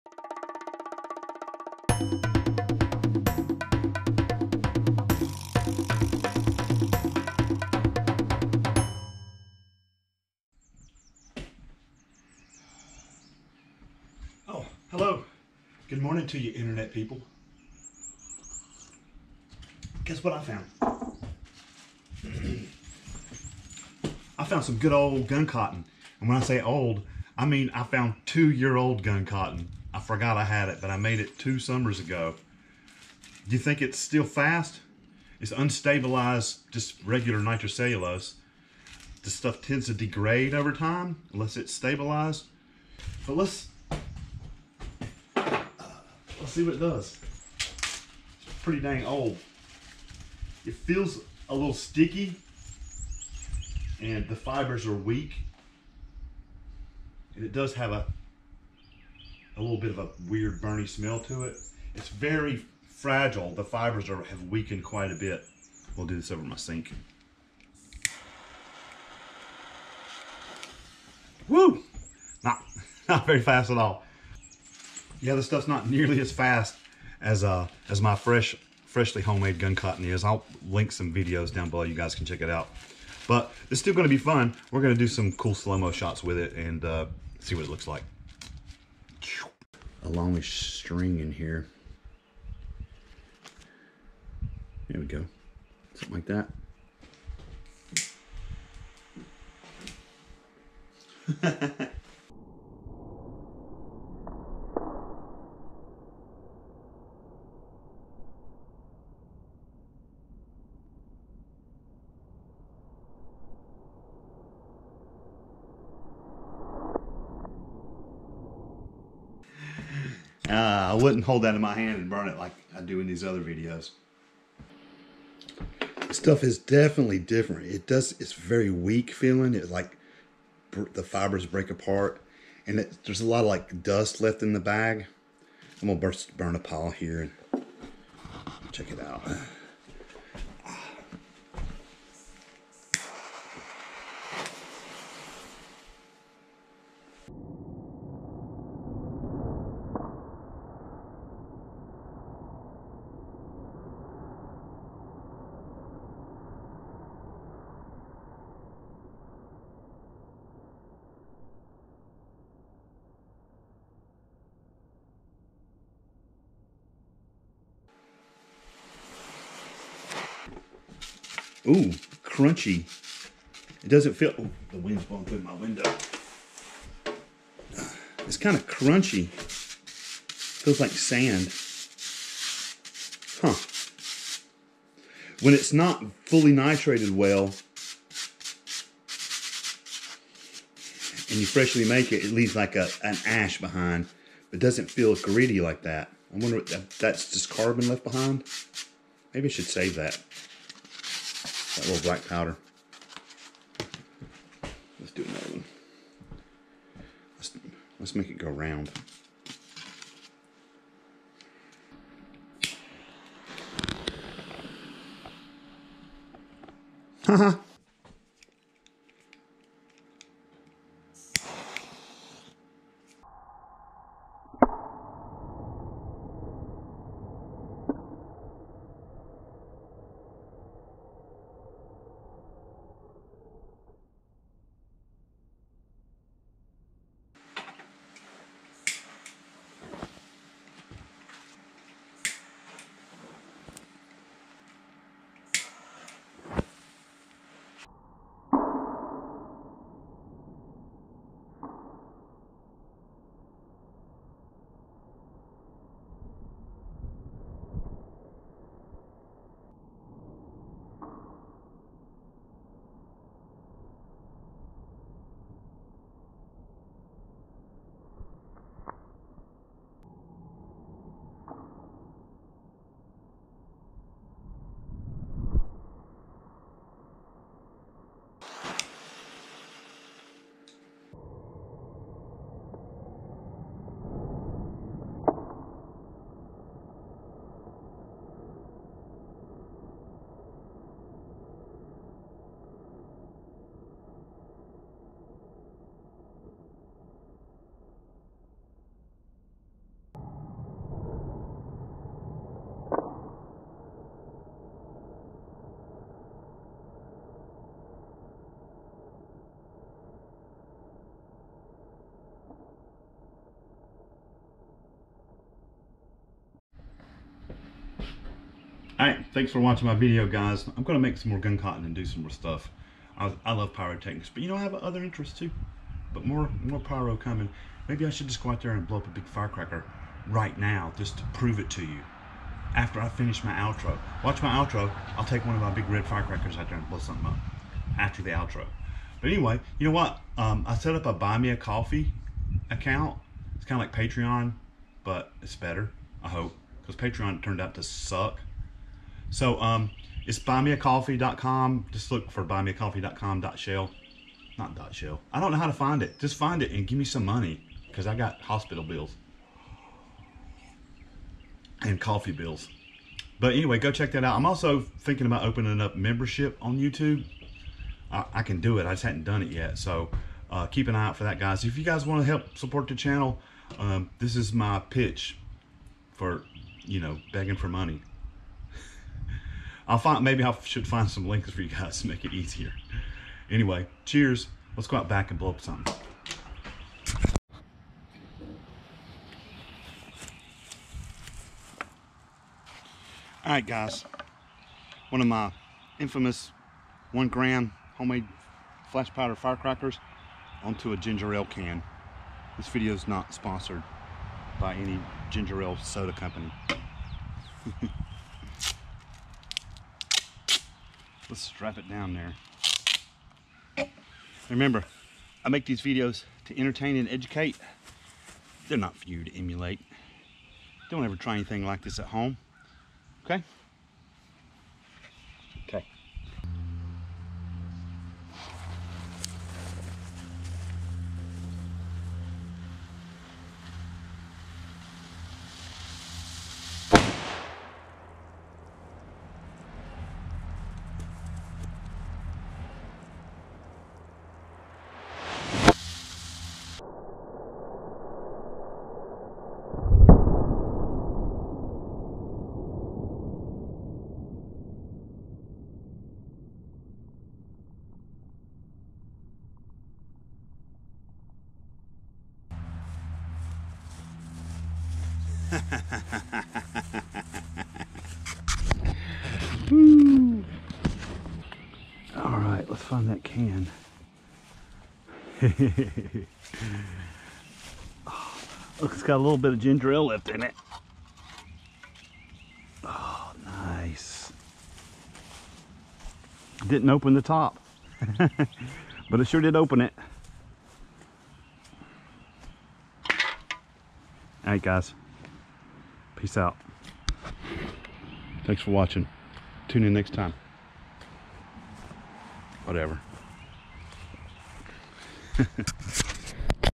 2 Year Old Guncotton - Will It Still Burn?
Let's find out how fast it burns!

WARNING:

I make these videos to entertain and educate,
They are NOT for you to emulate!
Dont ever try what you see here at home!